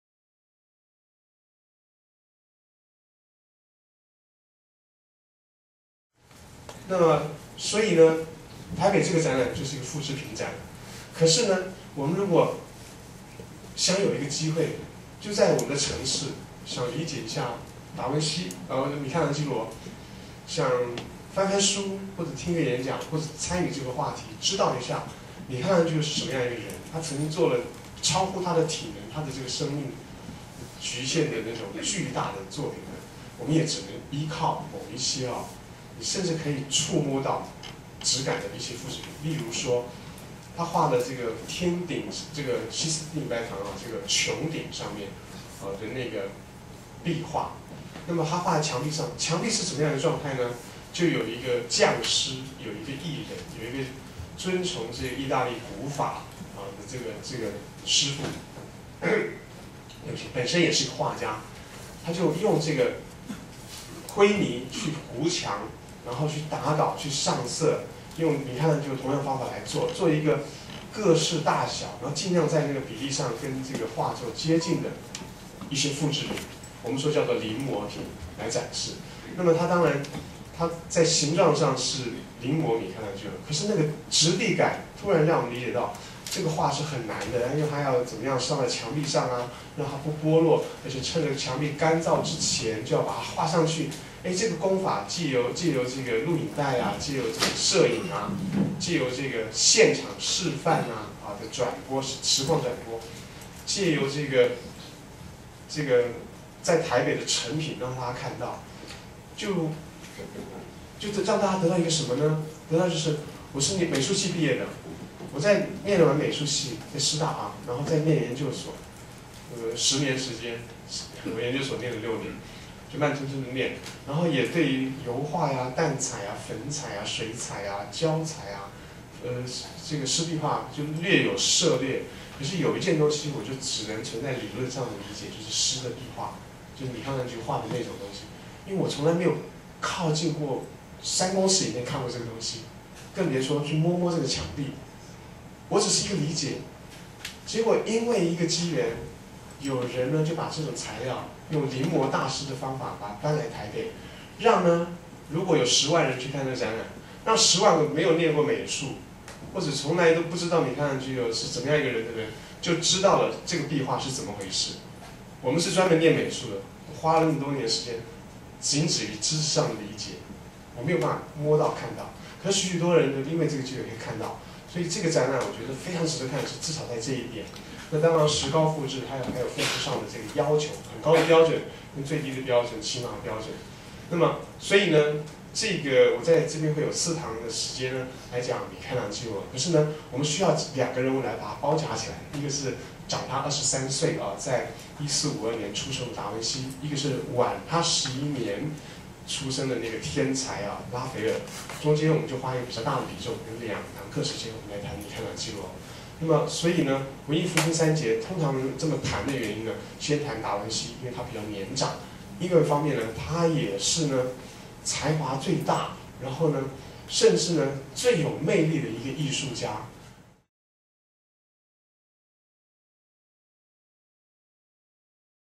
人文講堂【鄭治桂—文藝復興三巨匠】
隨著米開朗基羅特展即將於一月底展開至五月中，藉此機會回顧將文藝復興推向最高峰的代表人物：達文西、米開朗基羅、拉斐爾。
米開朗基羅始終承載著生命的激情飽含著豪雄的力量；
達文西是全才型「文藝復興人」的代表，畫作涵義多重，神秘幽深；
拉斐爾是畫家中的天使，他的繪畫總是令人感到和諧、明朗、崇高而優雅。
三位藝術大師不只擅用技法，更能突破形式，將他們對人性的看法、對人生的理解、對人類命運的思索，深刻表現在畫作上，流傳於世。